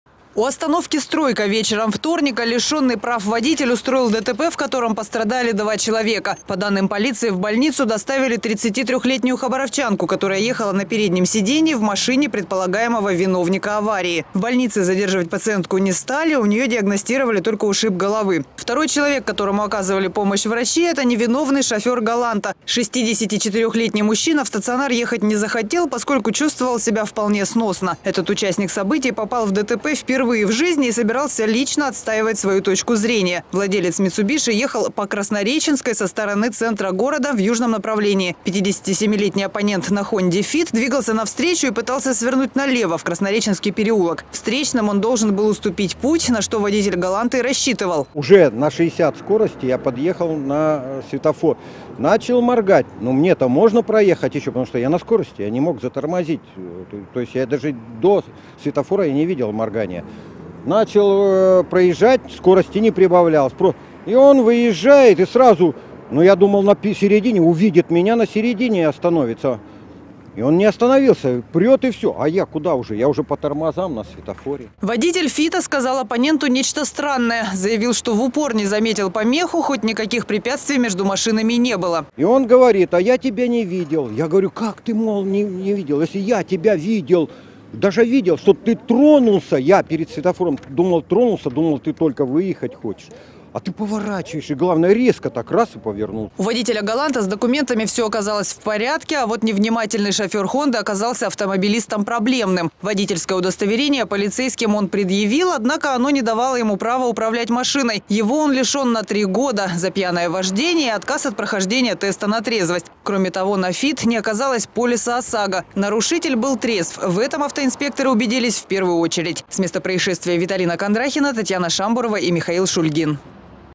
Лишенный прав водитель разбил иномарку хабаровчанина. MestoproTV
У остановки «Стройка» вечером вторника лишенный прав водитель устроил ДТП, в котором оказались травмированы два человека. По данным полиции, в больницу была доставлена 33-летняя хабаровчанка, ехавшая на переднем сидении в машине предполагаемого виновника аварии, сообщает портал «Губерния». Все последние происшествия вы можете найти в разделе "Место происшествия" на портале "Губерния"